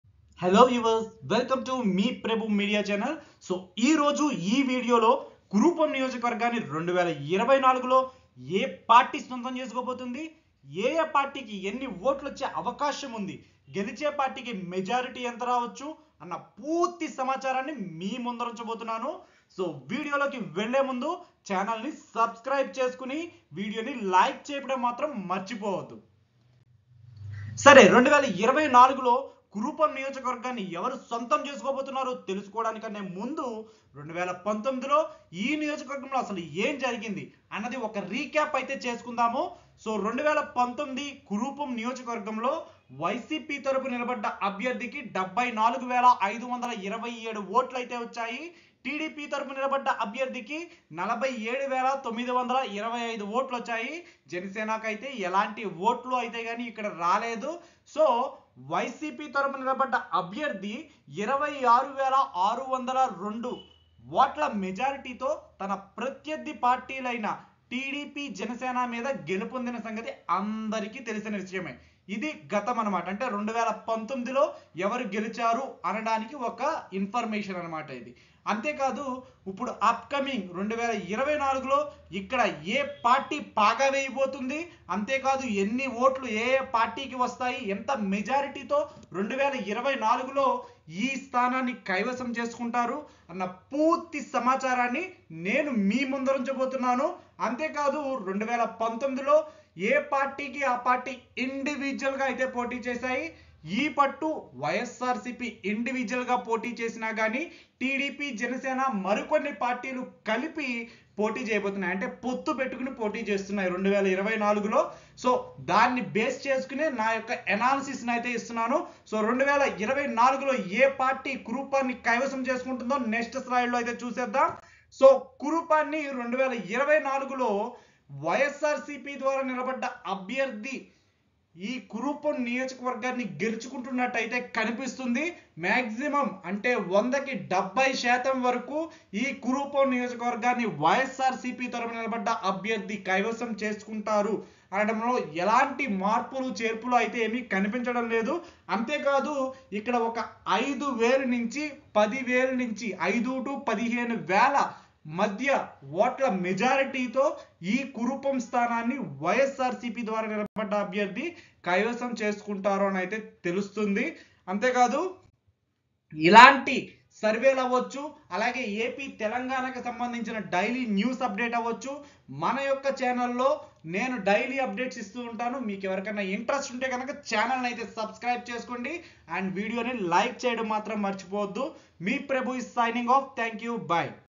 2024 Kurupam Election Survey: YSRCP, TDP, Janasena - Who Leads? Mee Prabhu Analysis
🔍 Dive into the heart of Kurupam's political landscape with our latest 2024 election survey analysis. In this comprehensive video, Mee Prabhu takes you through an in-depth examination of the current political scenario, focusing on the key players: YSR Congress Party (YSRCP), Telugu Desam Party (TDP), and Jana Sena Party (Janasena).

📊 We've meticulously gathered data and opinions from a wide range of sources to bring you a clear picture of the upcoming electoral battle. This video breaks down the complex dynamics and emerging trends that are shaping the voter sentiments in Kurupam.

🔑 Highlights of our analysis include:
► Voter trends and preferences in Kurupam
► Detailed insights into the policies and promises of YSRCP, TDP, and Janasena
► Impact of recent political developments on the electorate
► Expert opinions and predictions for the 2024 elections

🗣️ Join Mee Prabhu as he delves into the strategies and strengths of each party, providing a balanced perspective on their chances of leading in the 2024 elections. We also explore critical issues that are resonating with the voters and how they might influence the election outcome.

🌐 Stay informed and ahead in the political discourse. Whether you're a political enthusiast, a resident of Kurupam, or just keen on understanding the dynamics of Andhra Pradesh politics, this video offers valuable insights and analysis.

Your engagement helps us bring more such detailed content. Drop your thoughts and questions in the comments below - let's discuss what the future holds for Kurupam in the 2024 elections!